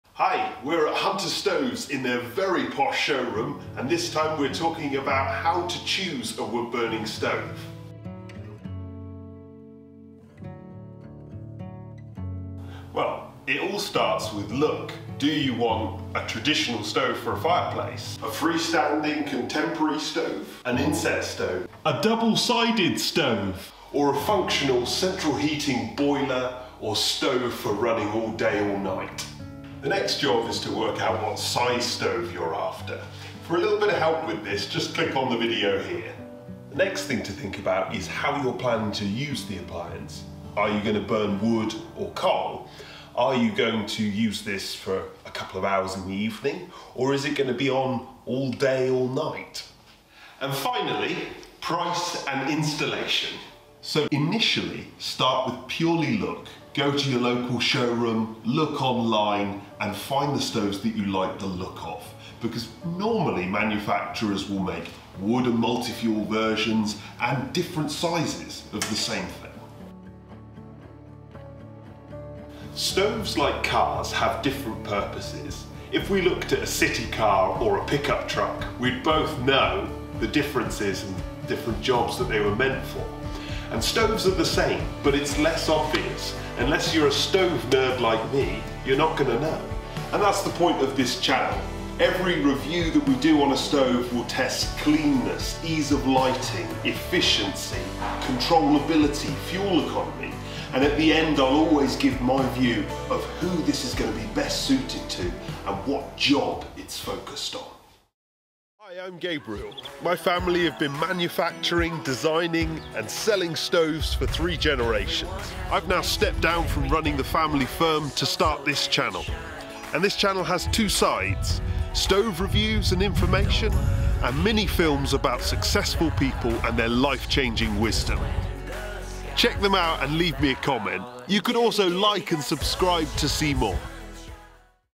How to choose a stove
This (I hope) is a really useful video to have seen when choosing a new stove. Most retailers try and sell what they want to sell, and because most people have so few stoves in their life they never necessarily know whether their stove is any good or not. Getting the right stove for your use and fuel has a huge impact on fuel economy and the environment, not to mention your enjoyment as well!